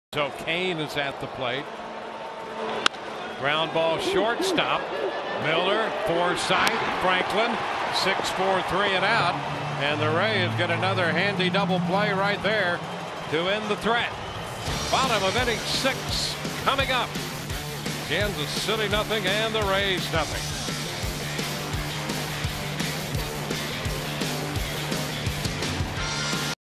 KC@TB: Miller starts double play to keep it scoreless
Brad Miller handles a Lorenzo Cain grounder at short and tosses it to second to start a 6-4-3 double play that ends the top of the 6th